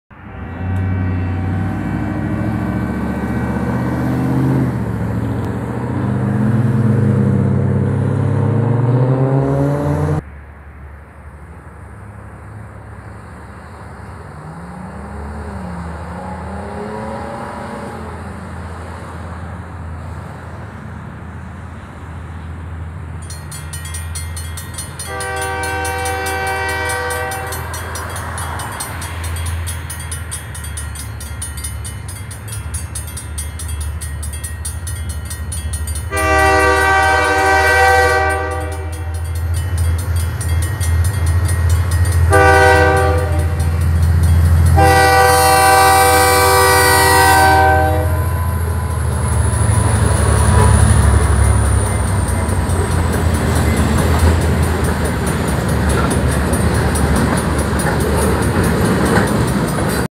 B&W video of Monday afternoon Portland & Western 2316 crossing SE River Road leaving Hillsboro
Portland & Western 2316(Albany) long hood forward crossing SE River Road passing MP 763.80 Tillamook District hauling 6 full centerbeams from Hampton lumber mill in Banks Oregon. 4 boxcars from International Paper in Beaverton Oregon. A Pacific Northwest Shortline Railroad Production ©️ 2020